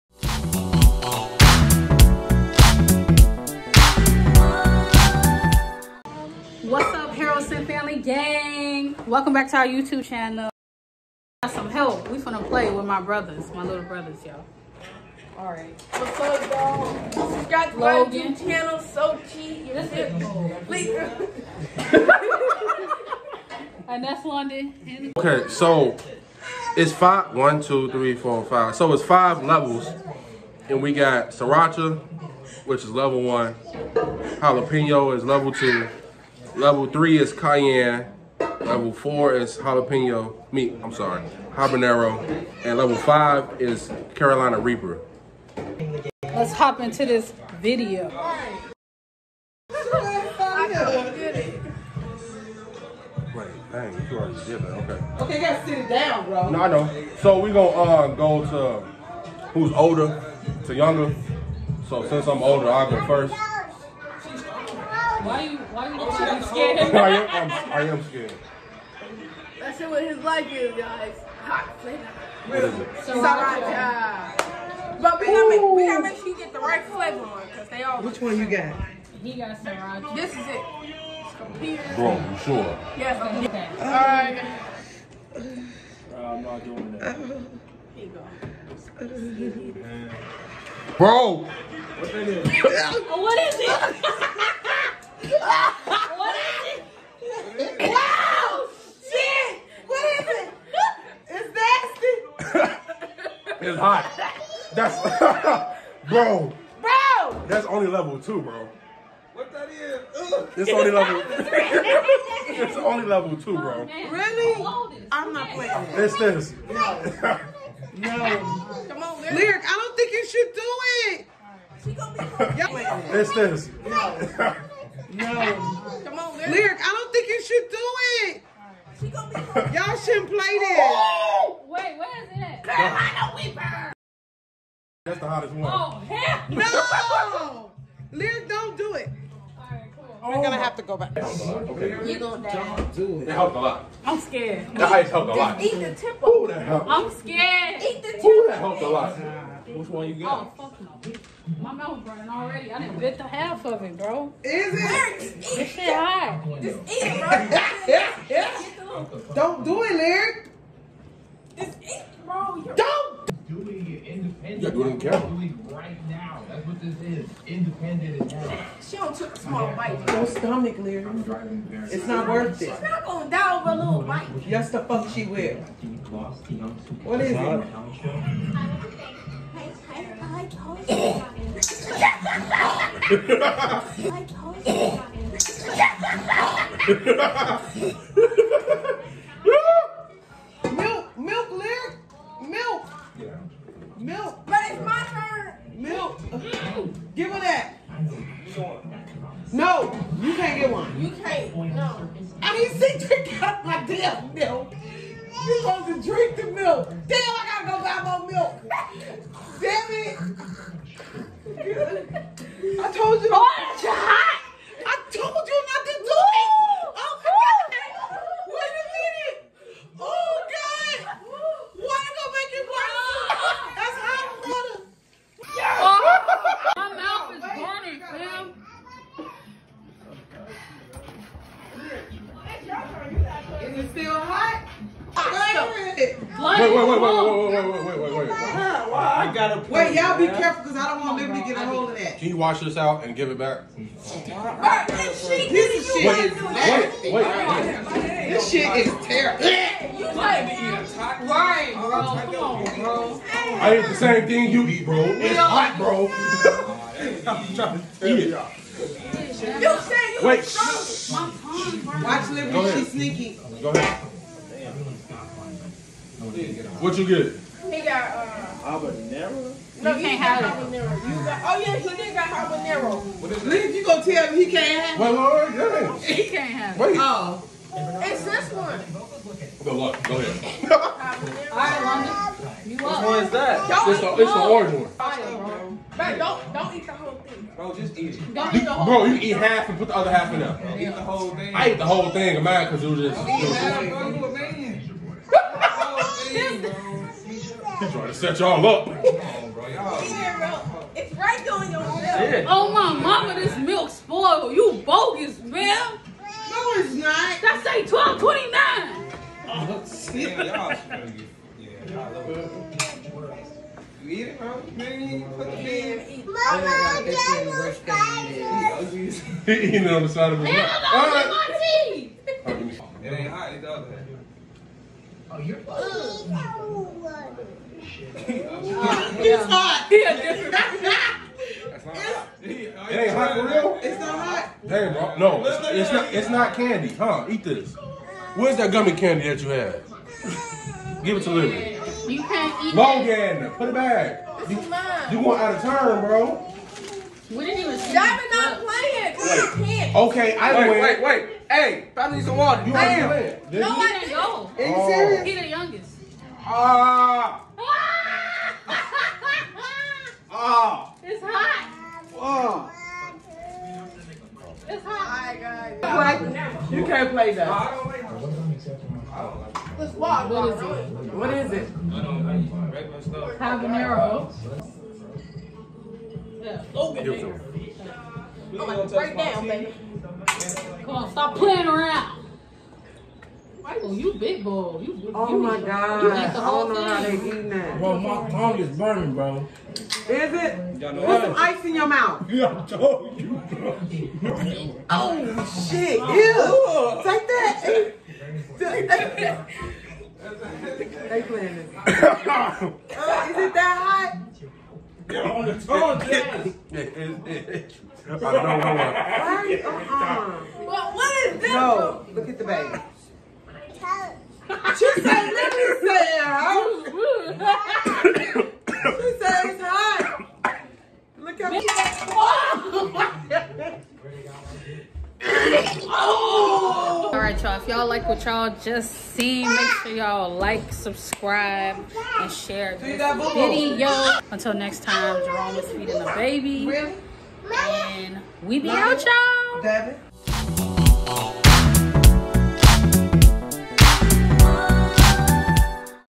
SPICY Bean Boozled Challenge! **Husband Throws Up!**
We decided to do the hot and spicy bean boozled challenge! Including our little brothers and our moms commentary (Lyric’s brothers and mom). Man, we got sick doing this challenge! A lot of laughs and jokes! We hope you enjoy this video!